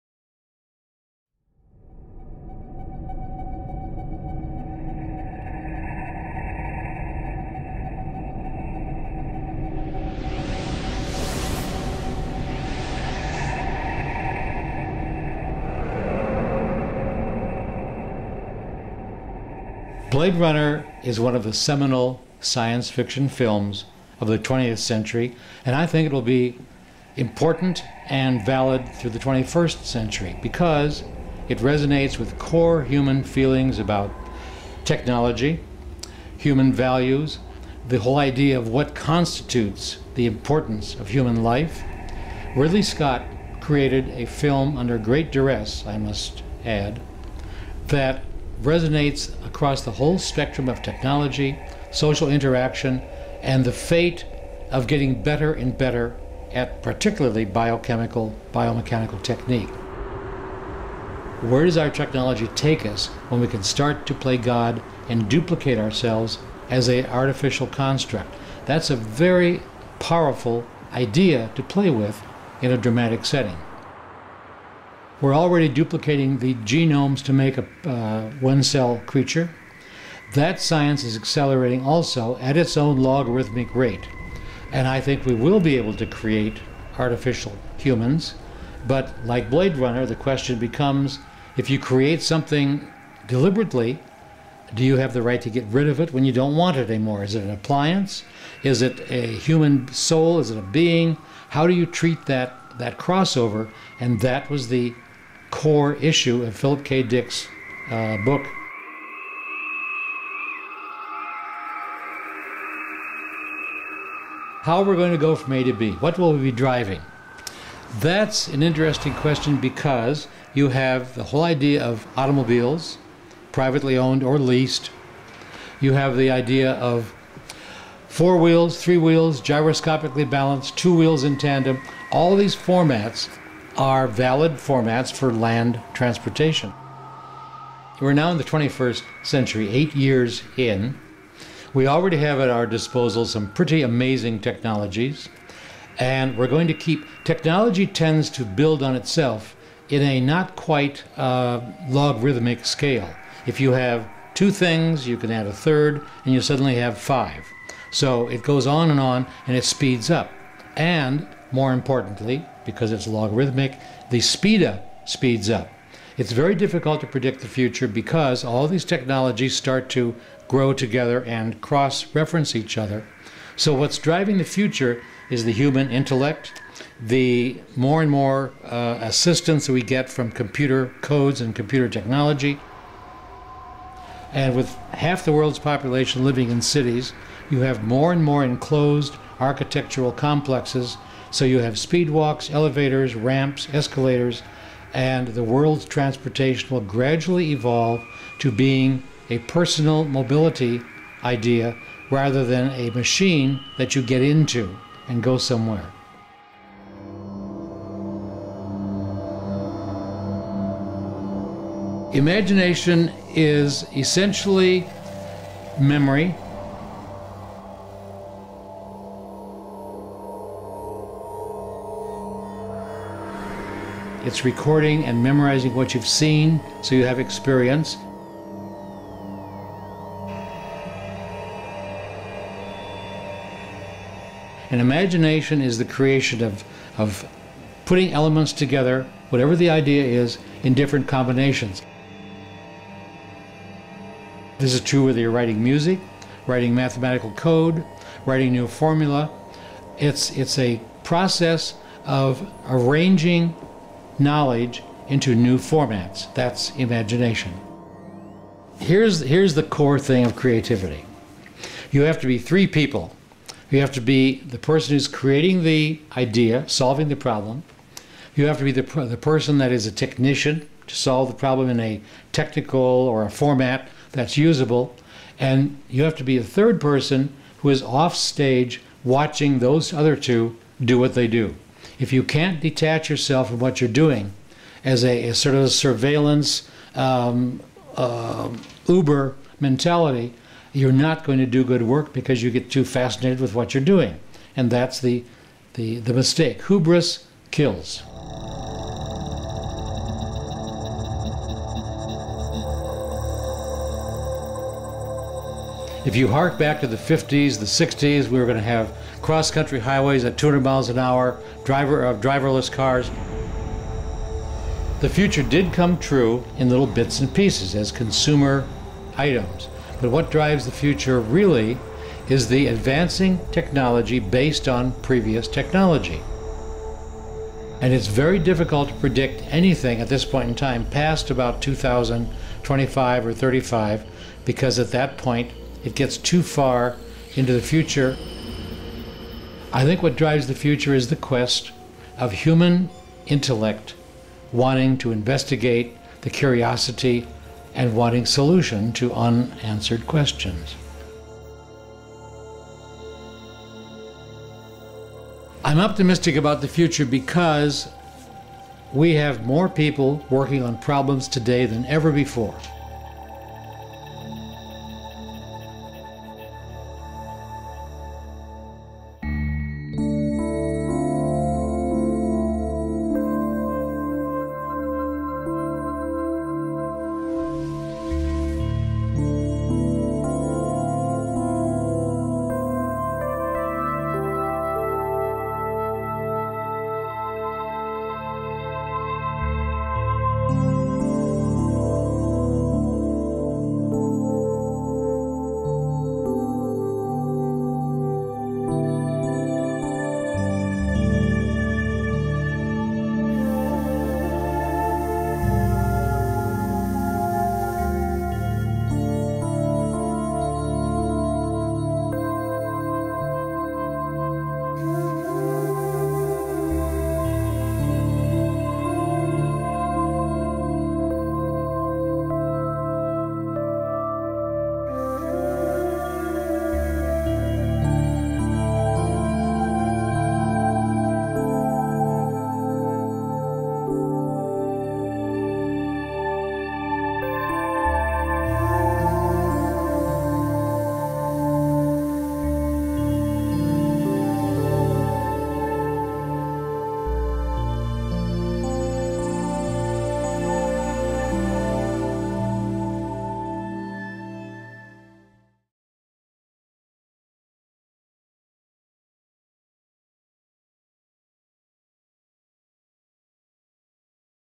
2019: A Future Imagined
Visual Futurist Syd Mead ("Blade Runner," "Aliens," "Tron") reflects upon the nature of creativity and how it drives the future. This featurette provides insight into the fascinating mind of one of the most influential artists of modern cinema and automotive design.